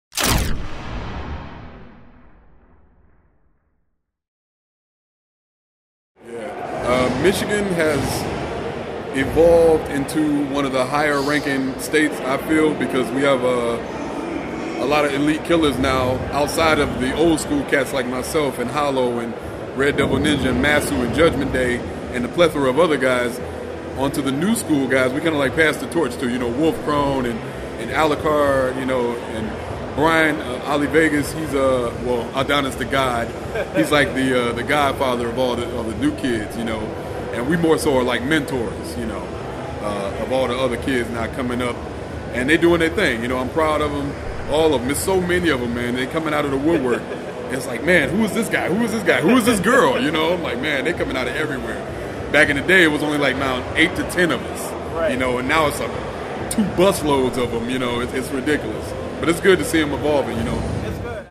PowerUp Quick Insight - VDO
VDO speaks on the new generation of players coming from Michigan!